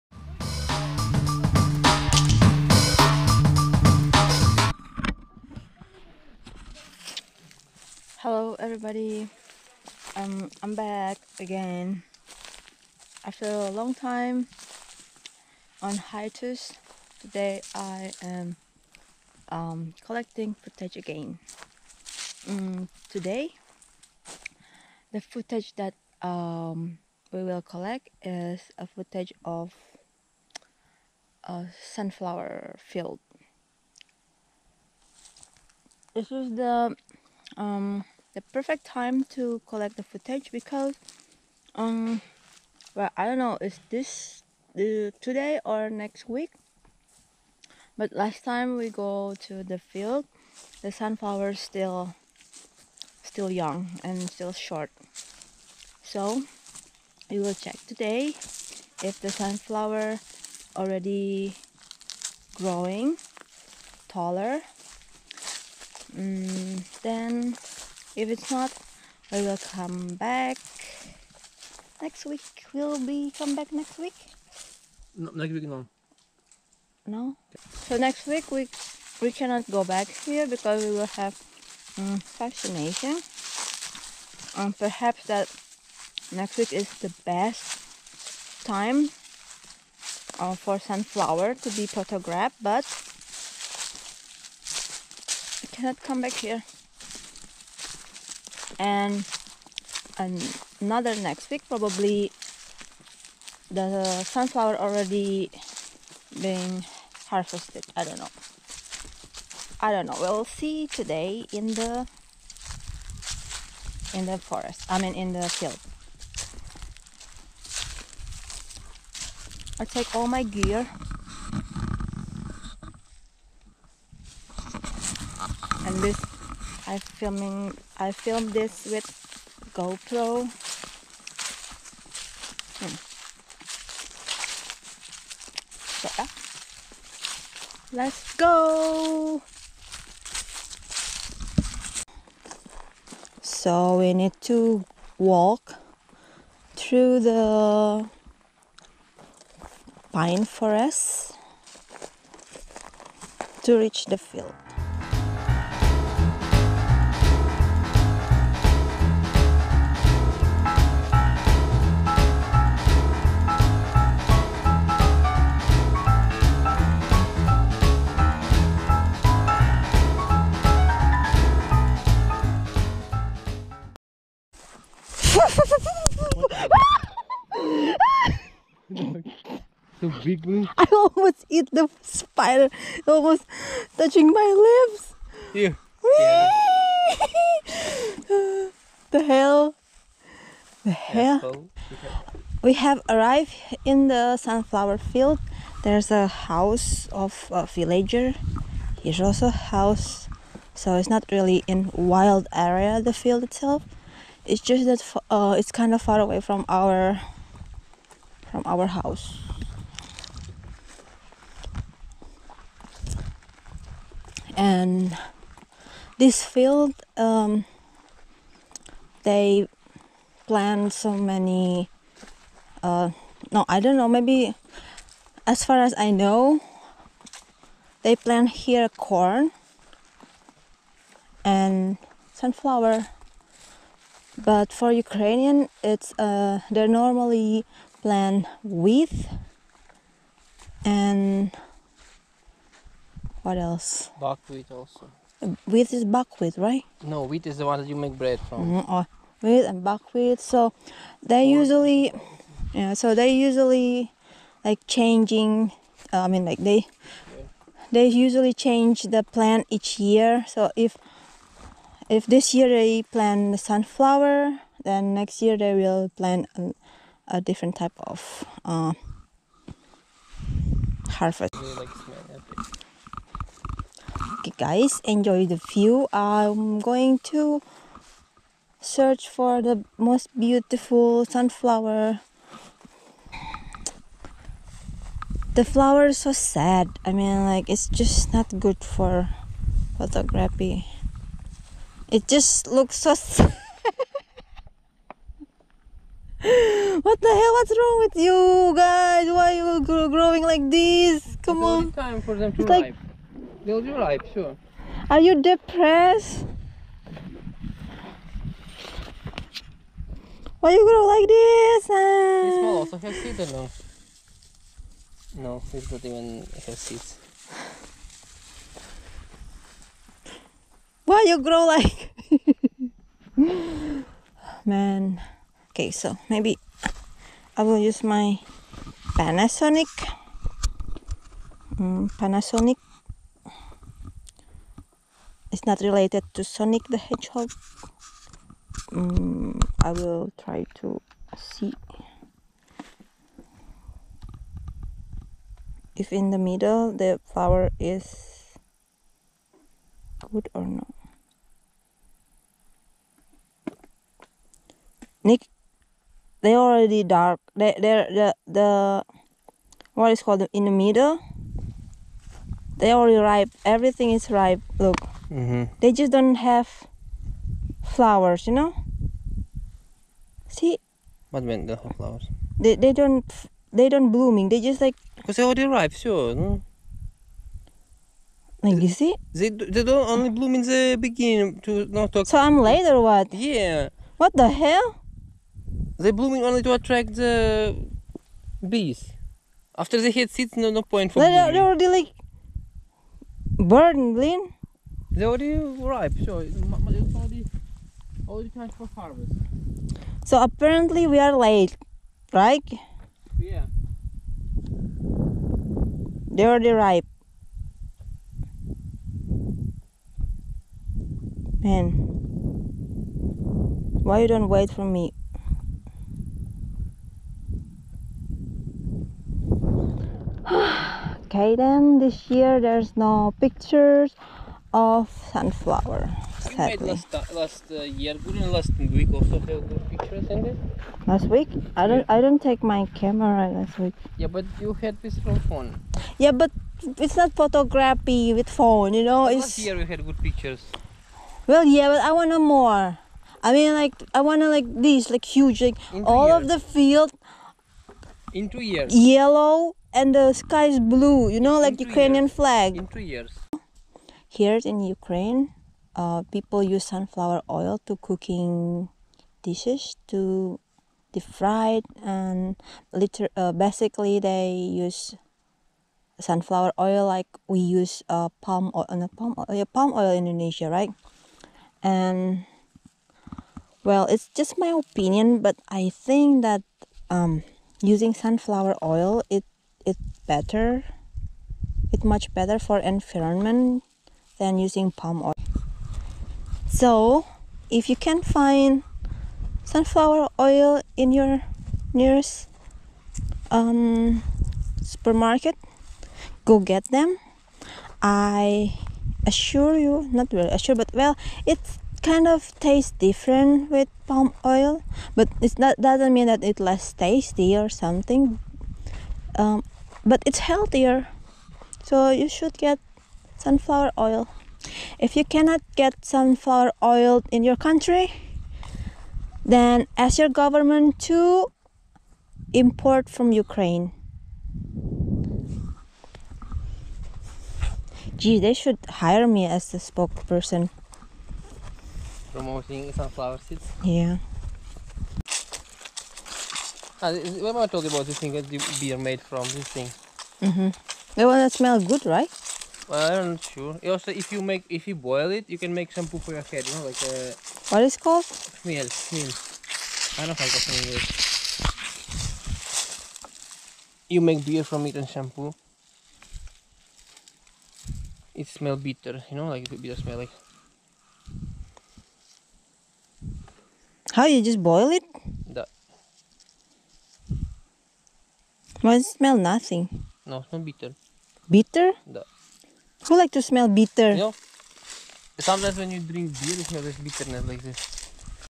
Visiting Sunflower Field
i came too late the sunflowers already riped
(ノ｀Д´)ノ彡┻━┻